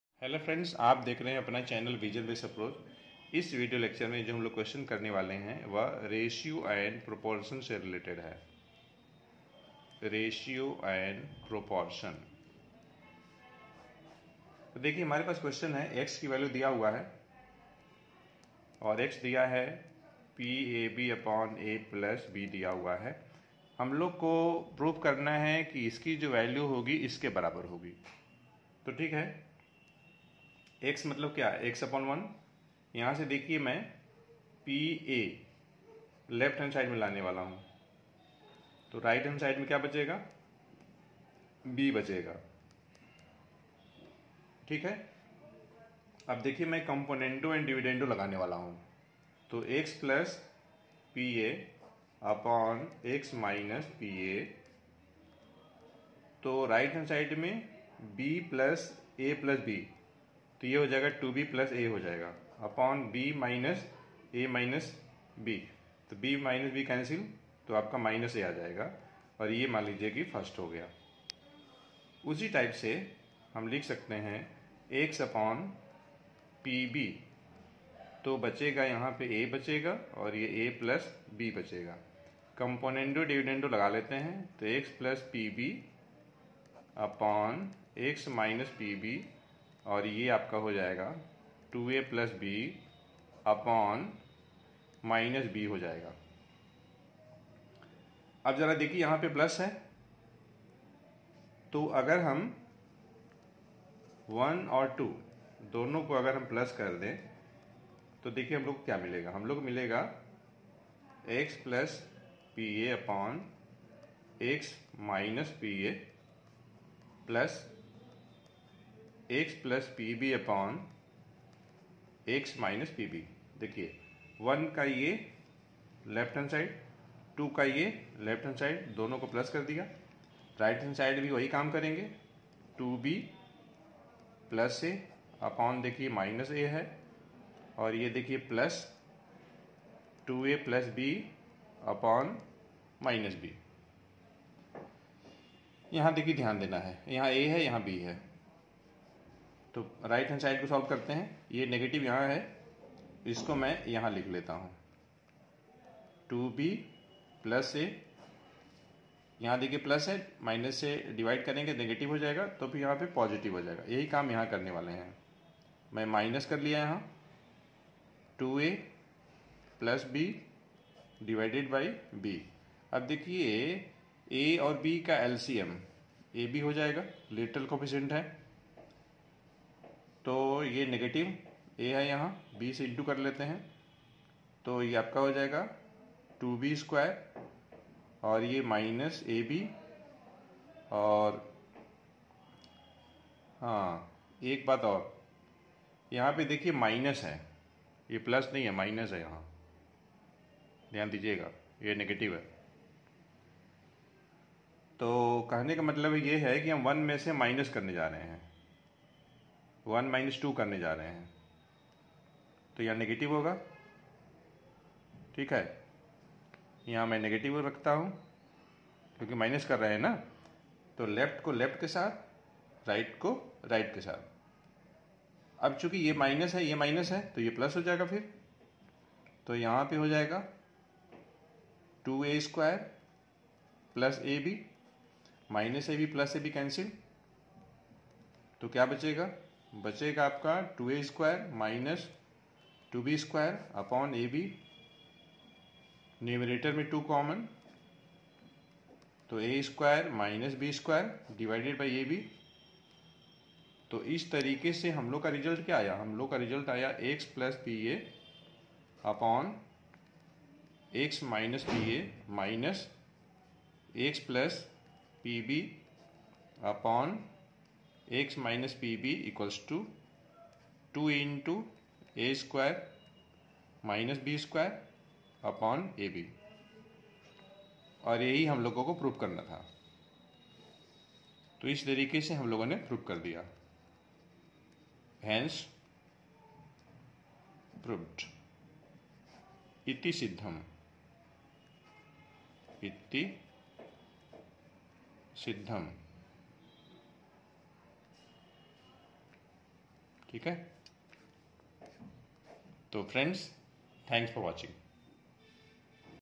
Ratio and Proportion Part 03
I hope 🤞🍀 that all of you have understood the topics which I have discussed in this video 📸 lecture. This video 📸 lecture covers Questions related to Ratio and Proportion.